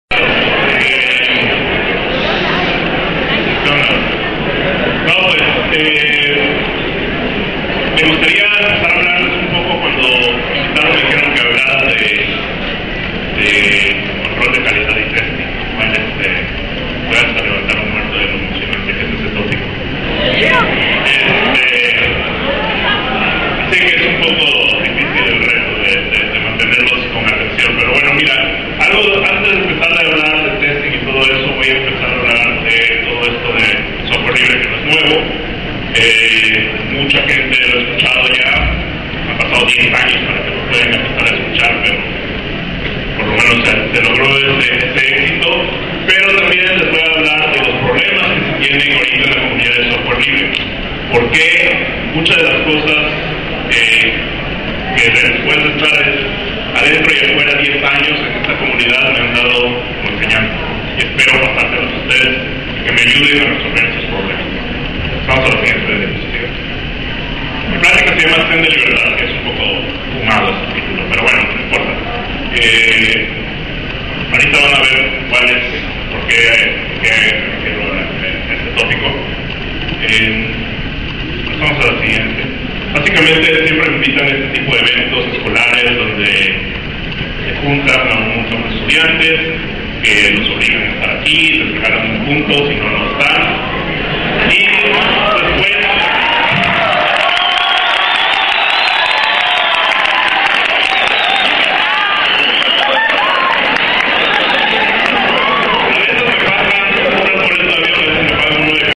Platica de JZA en Coatza
Pequeña parada en Coatzacoalcos para hablar de Software Libre y control de calidad y testing, como se prueba el software y como lo generamos.